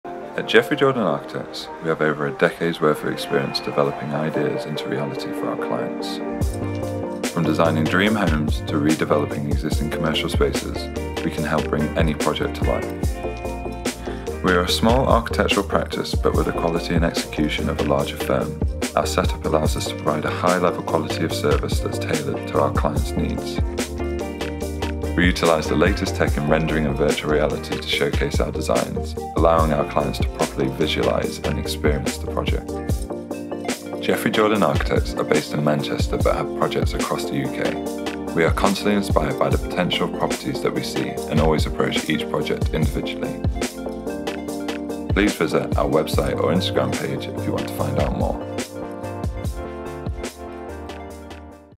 Jeffrey Jordan Architects (ad)
A close friend at @jeffreyjordanarchitects tasked us to produce a quick little "hello" video to show them off to the world. A fun little day filming around their sites in Manchester and a lot of fun compiling it together made these 52 seconds.